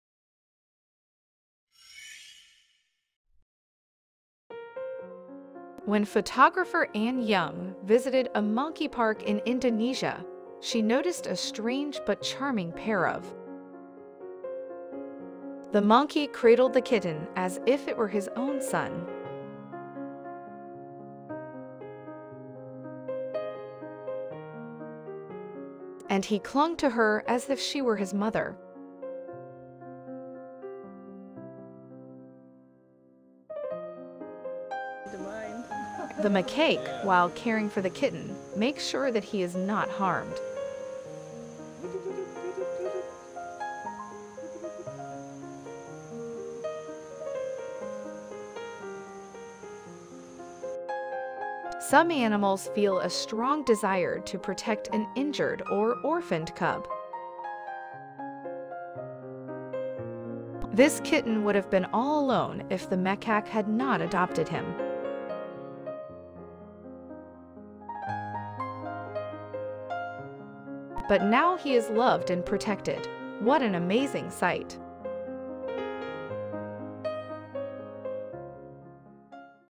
Monkey Finds Stray Kitten And Cuddles Him Like It’s Her Own Baby
When photographer Anne Young visited a monkey park in Indonesia,
 she noticed a strange but charming pair of. The monkey cradled the kitten as if it were his own son. And he clung to her as if she were his mother.